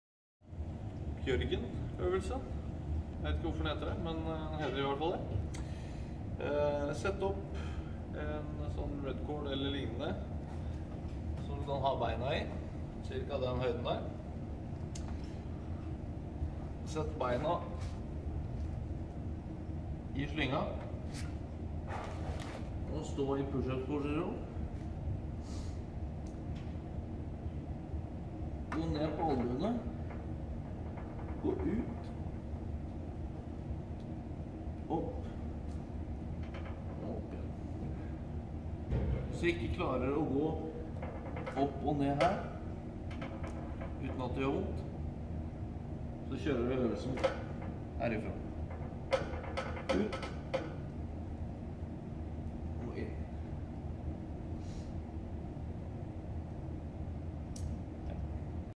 Core Bjørgen øvelse
Husk å stabilisere hofte og ryggsøyle. Ved å ha beina lavere og lenger til høyre vil belastningen blir større.